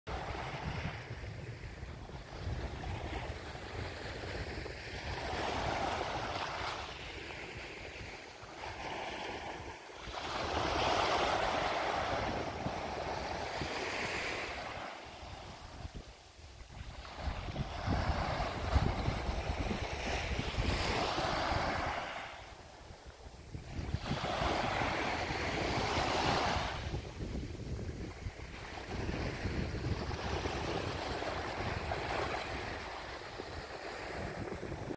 (VIDEO) Ogliastra, l'affascinante spiaggia di Cea: la struggente bellezza anche d'inverno
Sabbia bianca finissima, mare color smeraldo e i caratteristici faraglioni di porfido rosso che si ergono dall’acqua: sarà una visione che resterà scolpita nell'anima per sempre. Parliamo dell'incantevole bellezza della spiaggia di Cea, un lembo di paradiso terrestre ubicato tra il territorio di Tortolì e Bari Sardo.